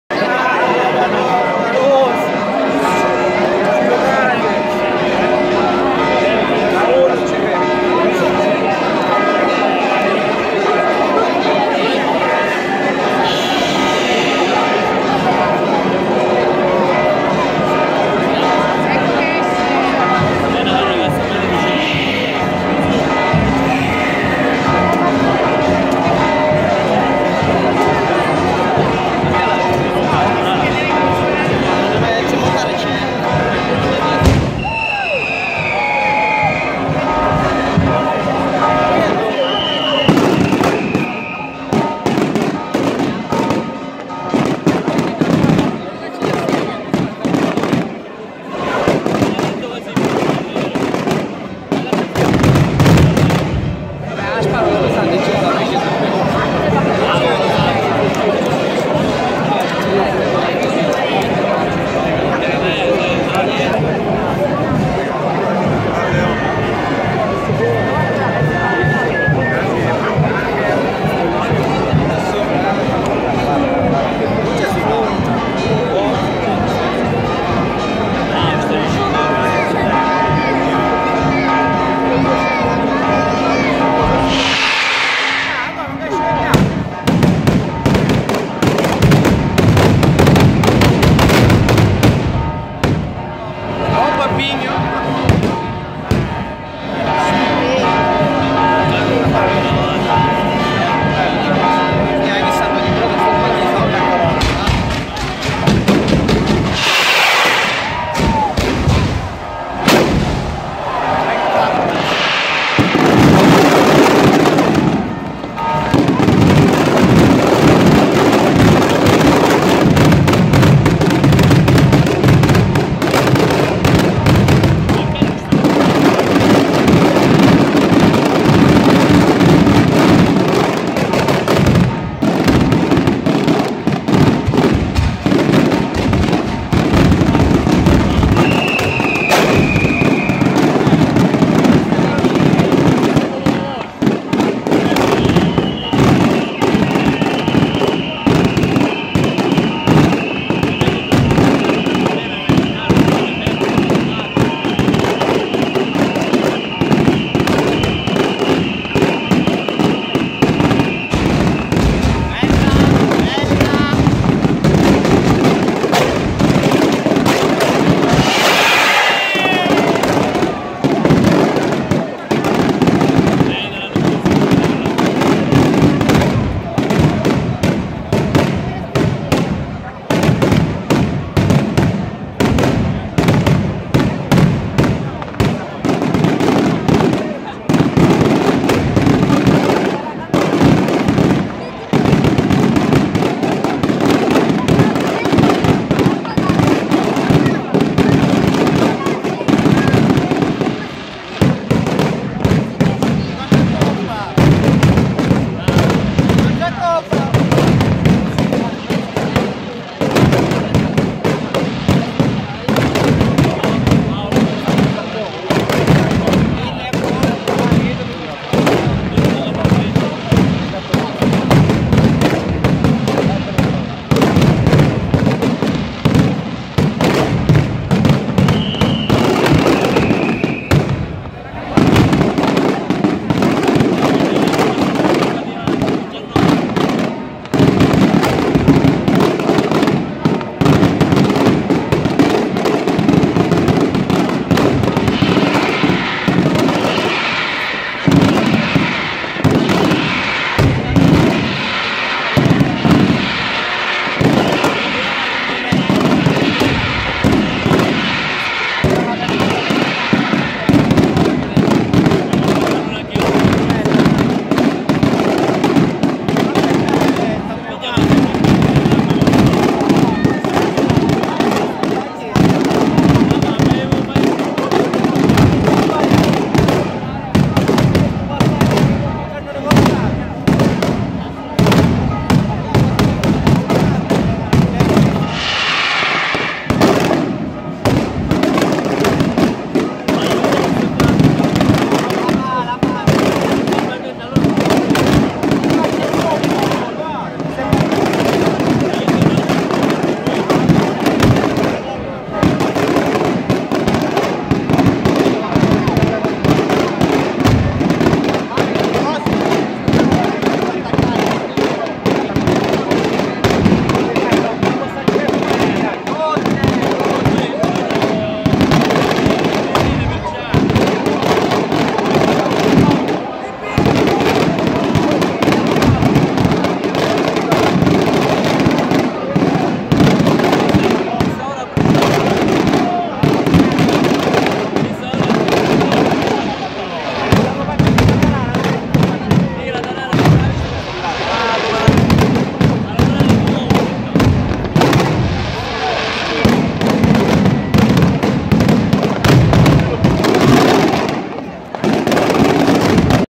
"Sciuta" San Sebastiano a Palazzolo Acreide 2019 video by Tano Licitra
video sulla tradizionale "sciuta" di San Sebastiano a Palazzolo Acreide (Sr)